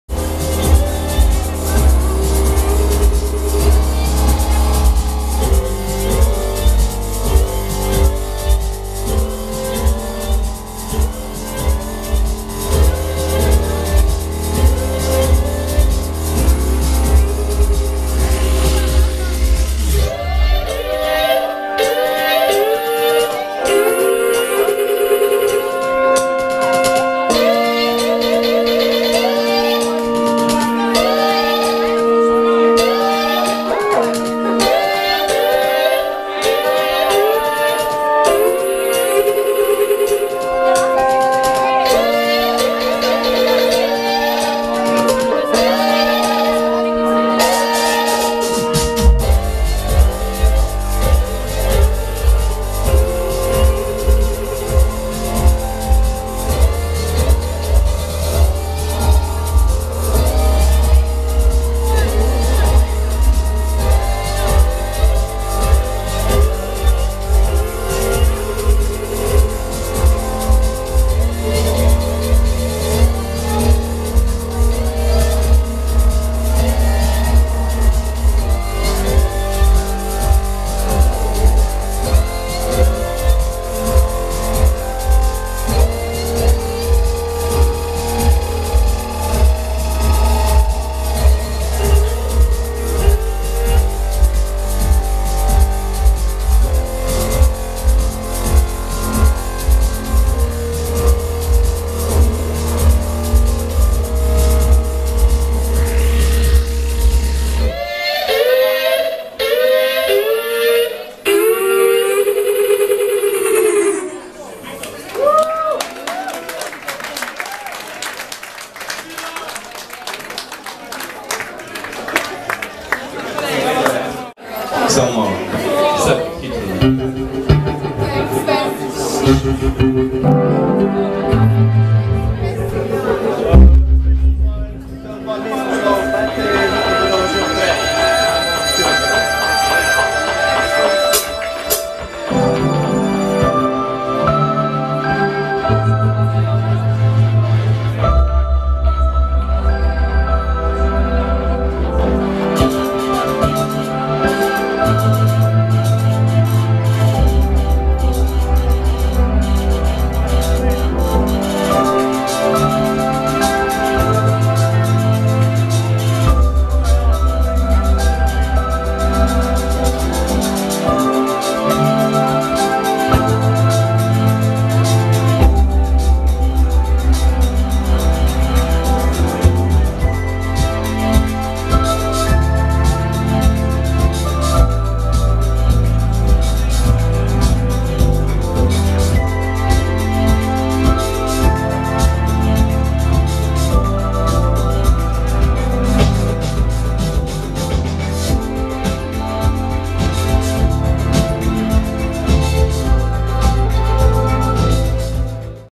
Kleemar @ Grossmann film and wine fest
26. julij, Ljutomer, Bar Klub